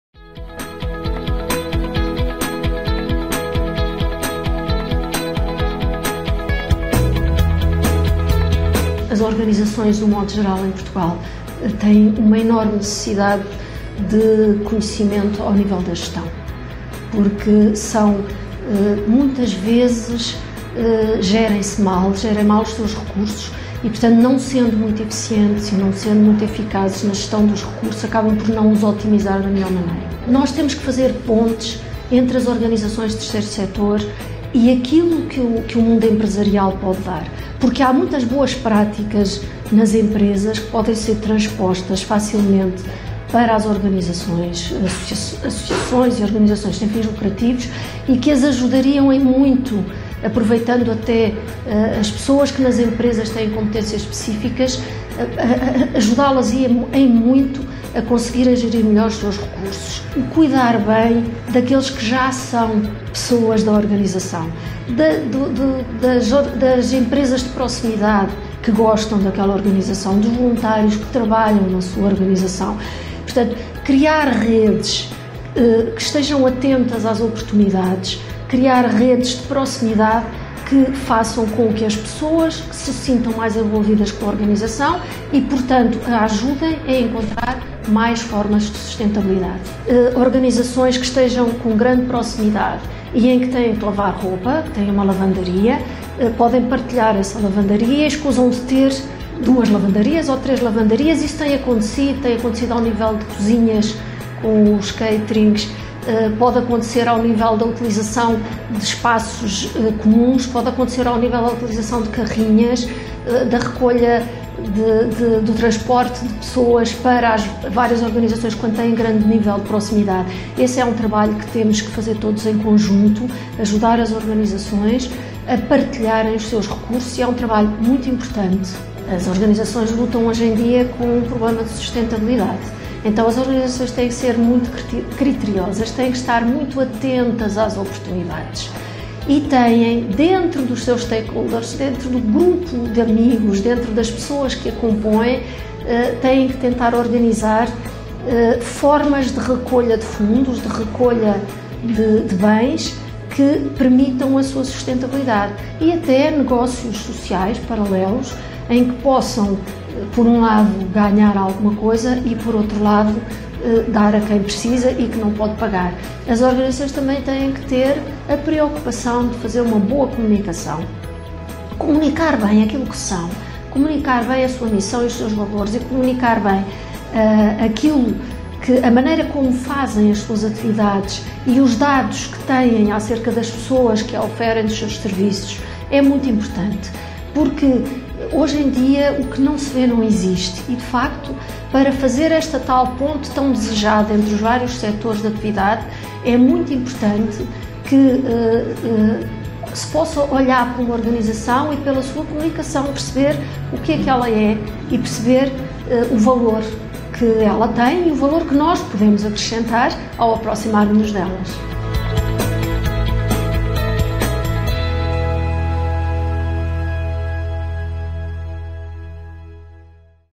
Ei TV Solidário - Apoio a instituições do Setor Social
A Entrajuda disponibiliza serviços de apoio a instituições do Setor Social. O Ei conversou com Helena Presas, coordenadora da Bolsa de Voluntariado da Entrajuda.